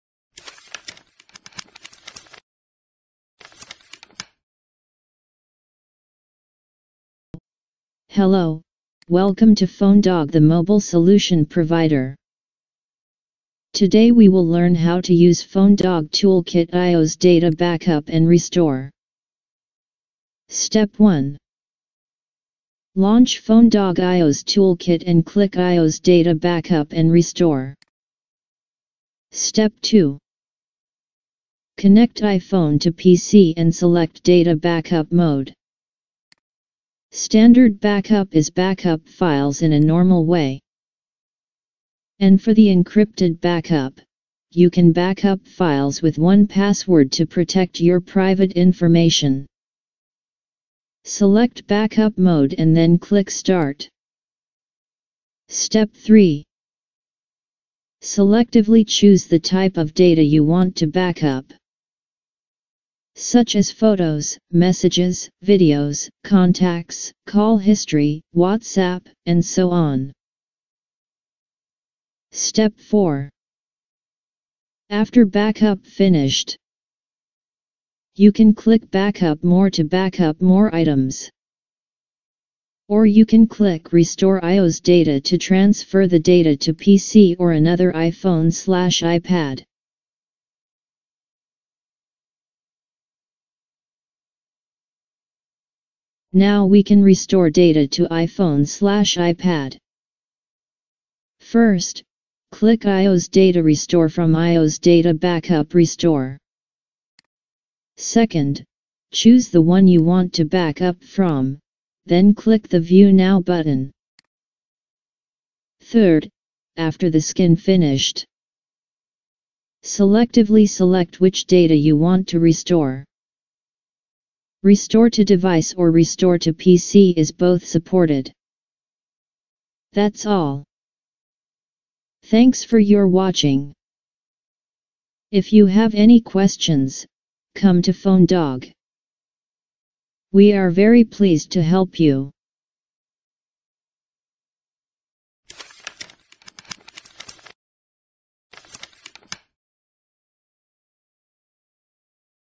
FoneDog Toolkit - iOS Data Backup & Restore
FoneDog - iOS Data Backup & Restore allow you to selectively backup and restore your iPhone/iPad data with One Click.
Step 1.  Launch FoneDog iOS toolkit and click iOS Data Backup and Restore
Step 2.  Connect iPhone to PC and Select Data Backup Mode.
“Standard backup" is backup files in a normal way.
And For the Encrypted backup, you can backup files with one password to protect your private information.
Select Backup mode and then click Start.
Step 3. selectively choose the type of data you want to backup. Such as Photos, Messages, Videos, Contacts, Call History, WhatsApp and so on.

Step 4. After backup Finished. You can click "Backup More" to backup more items.
Or you can click "Restore iOS Data" to transfer the data to PC or another iPhone/iPad.

Now we can Restore data to iPhone/iPad.
First, Click iOS Data Restore from iOS Data Backup & Restore.
Second, Choose the one you want to back up from, then click the "View Now" button.

Third, After the scan finished. Selectively select which data you want to restore. Restore to Device or Restore to PC is both supported.